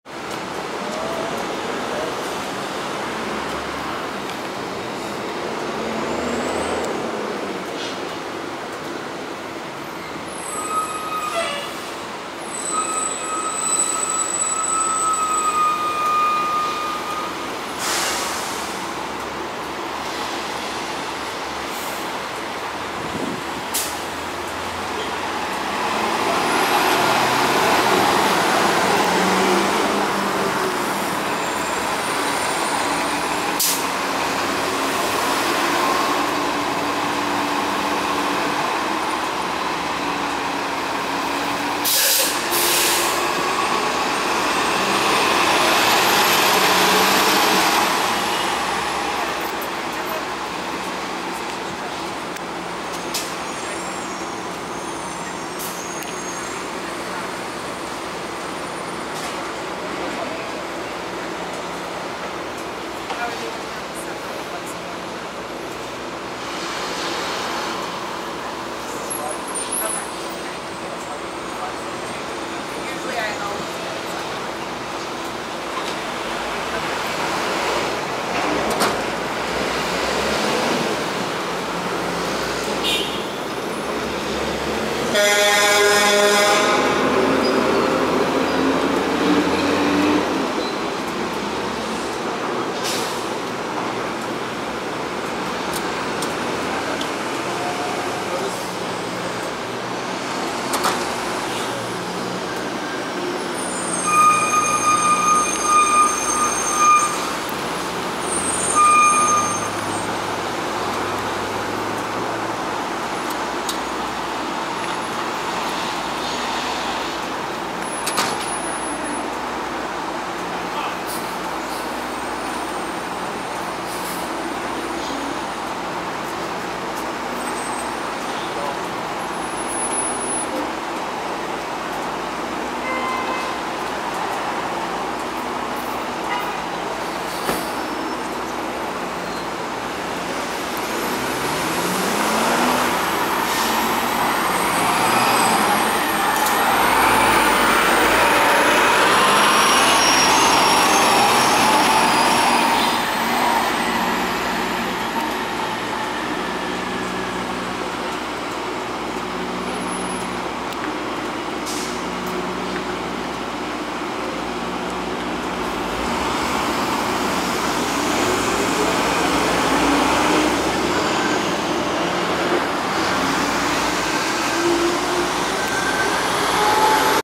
MaBSTOA: Xcelsior & Galaxy Turning At Houston Street
Taken on 6.5.17 at Allen & East Houston Streets. Shown are:

 - New Flyer XD60 (Xcelsior) # 5067 on a South Ferry-bound M15 Local
 - New Flyer D60HF # 5624 on a Pike-Cherry Street-bound M15 Local